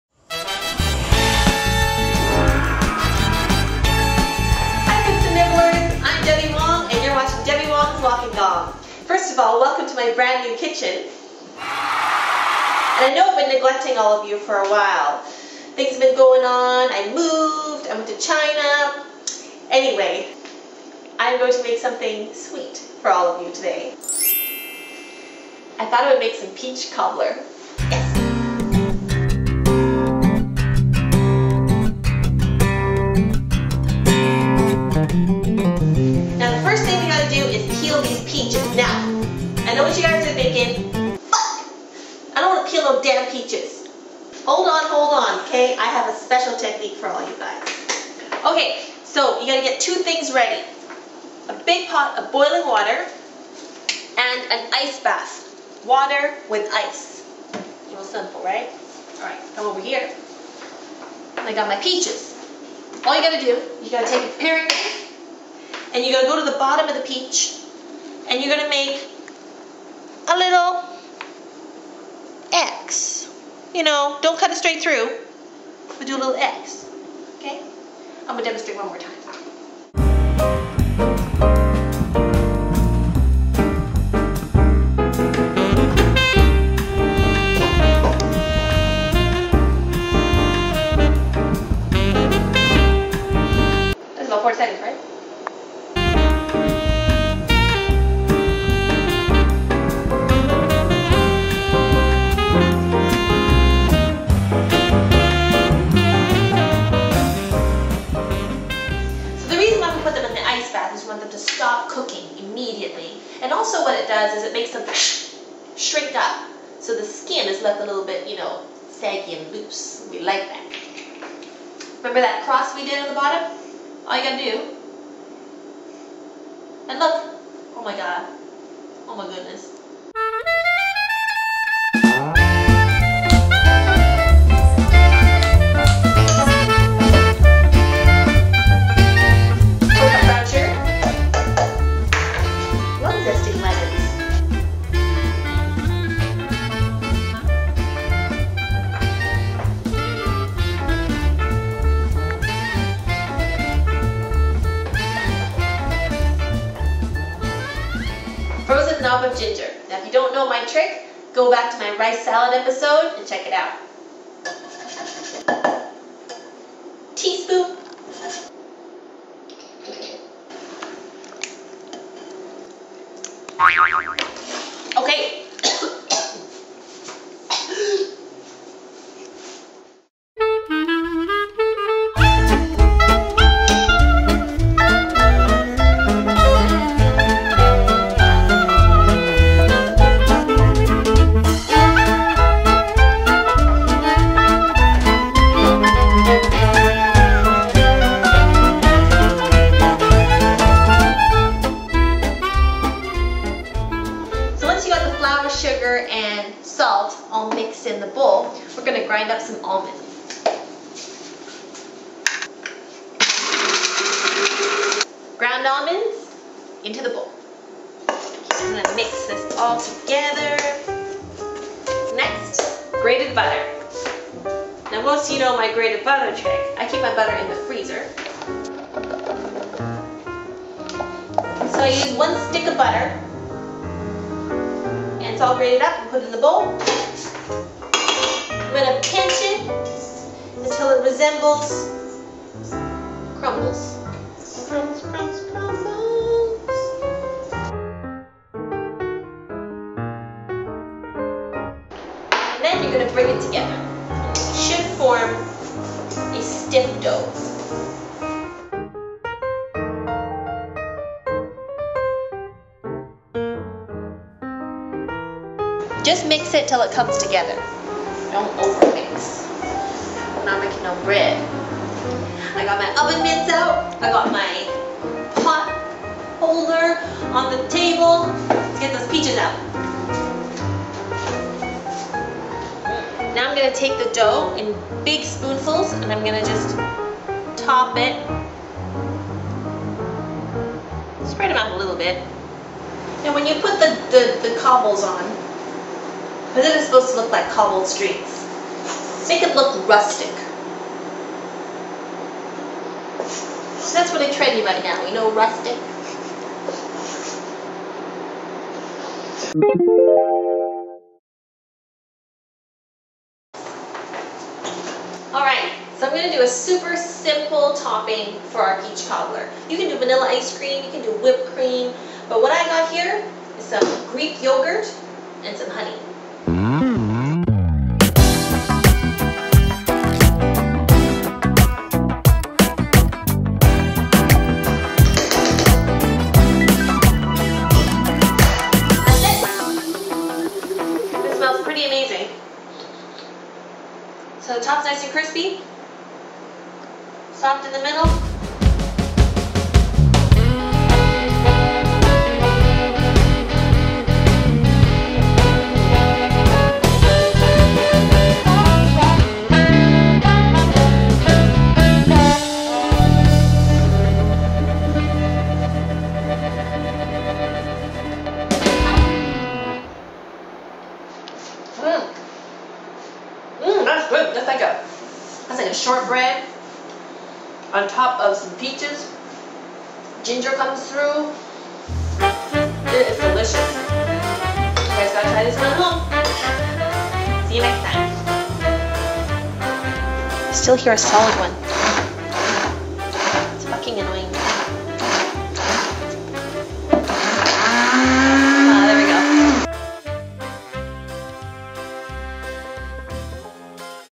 Almond Scented Peach Cobbler I Debbie Wong's Wok and Gong
A sweet dish from the south using last of the season peaches! Wong it.